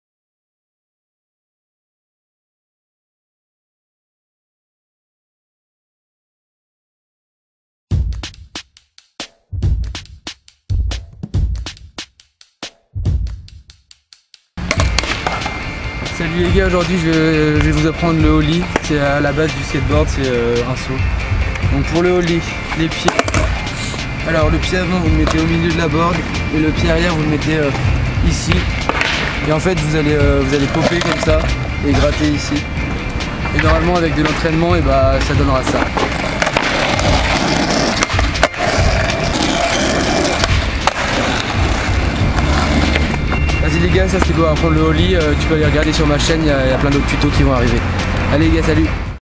Tutorial skate | Le Ollie
Aujourd'hui on vous apprend à faire le Ollie en skate. 🛹🔥
✅1 Vidéo par semaine le DIMANCHE à 18 heures.

✋N'hésitez pas à faire vos retours en commentaires (idées de vidéos, améliorations).